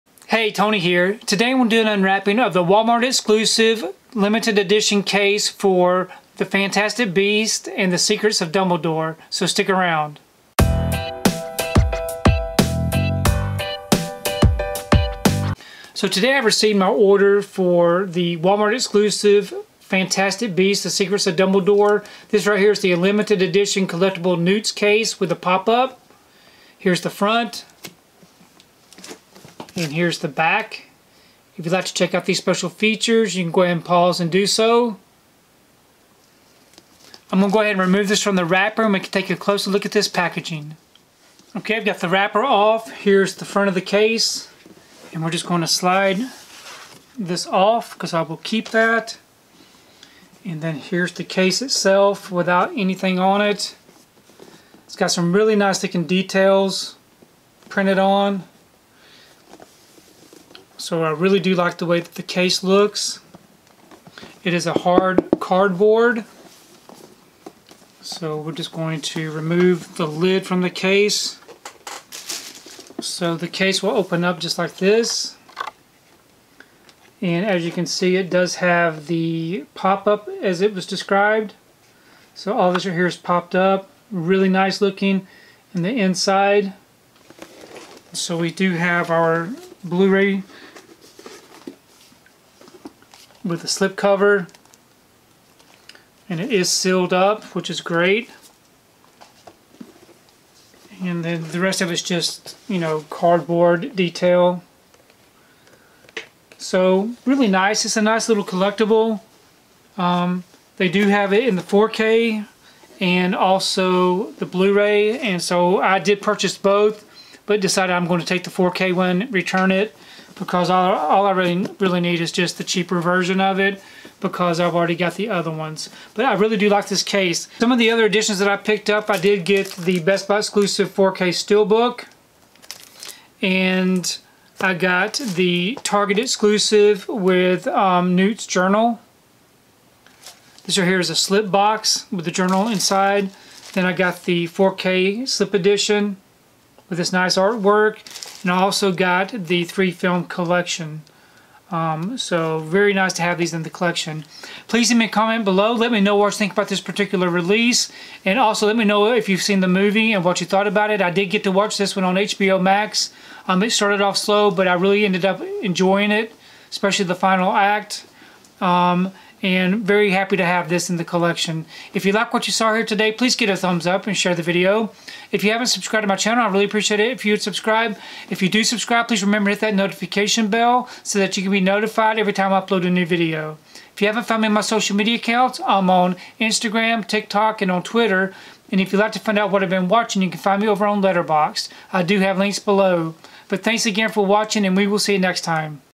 Fantastic Beasts The Secrets of Dumbledore Walmart Exclusive Limited Edition Collectible Newt's Case
Check out the Walmart Exclusive Limited Edition Collectible Newt's Case Exclusive Blu-ray Packaging for Fantastic Beasts The Secrets of Dumbledore!

► Add the 4K Ultra HD to your collection
► Add the Blu-ray Combo to your collection
► Add the 3-Film Collection to your collection